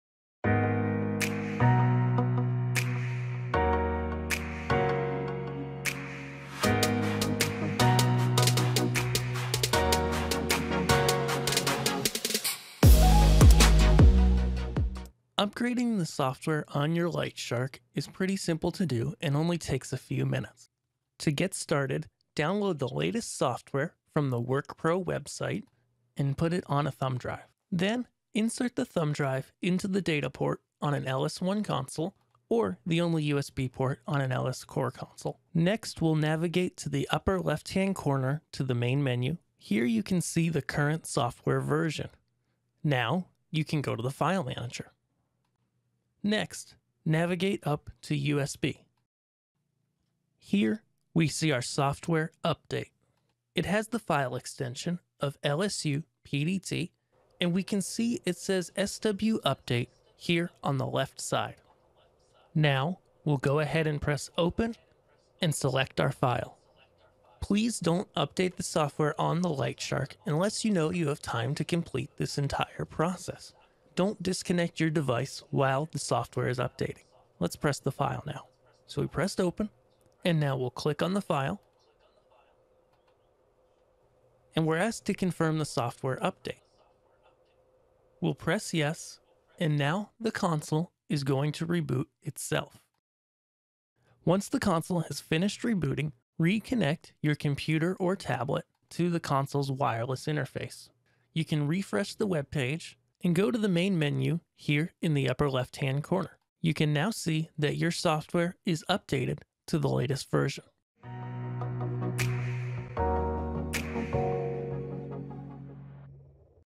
LightShark 9 Upgrading Software
Update the latest version of Lightshark with this easy tutorial